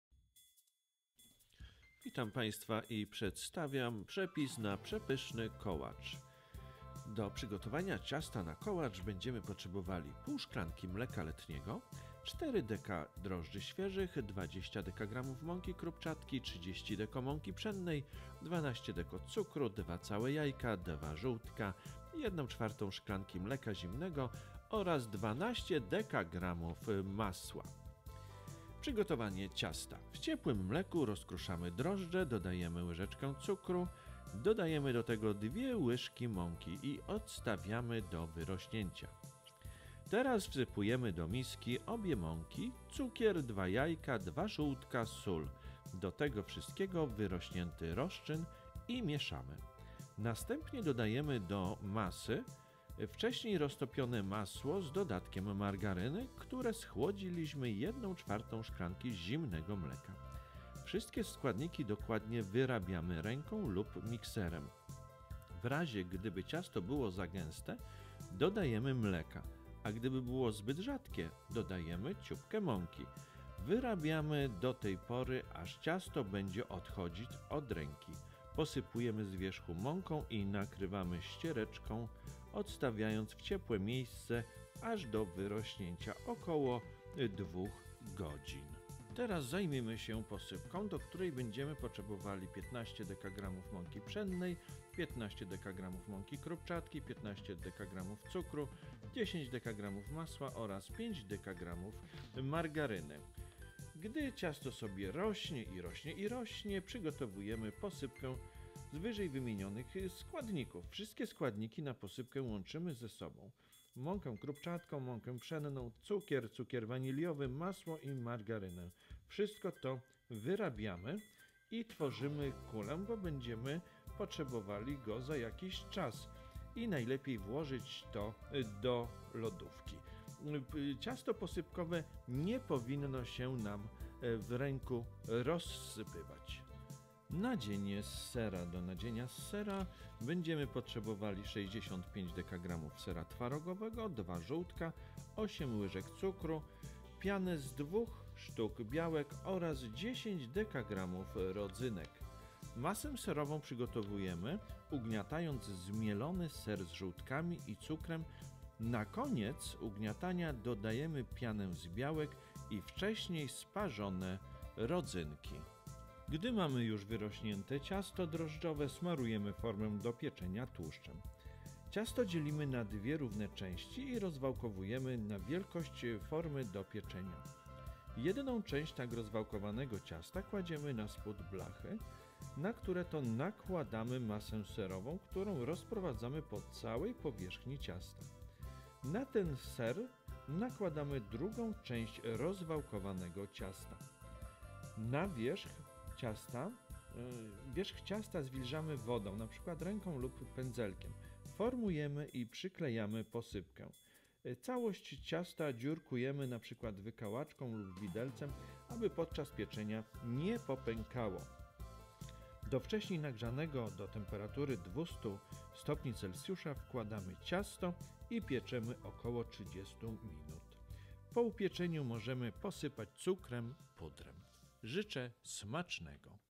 Kołocz ,kołacz z serem
Kołocz śląski -- nazwa zwyczajowa placka popularnego na Górnym Śląsku.
Występuje w czterech najpopularniejszych odmianach: kołocz z posypką (bez nadzienia), kołocz z nadzieniem serowym, makowym i jabłkowym